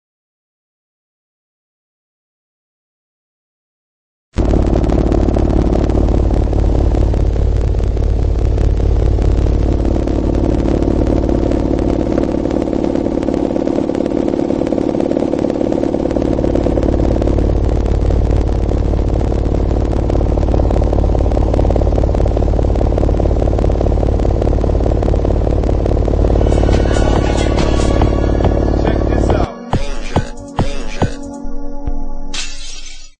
12vdb.com - car audio database: Orion Hcca122.1
Rob Fatts` (2) 12 HCCA12.2 free air, powered by a QSC MX1500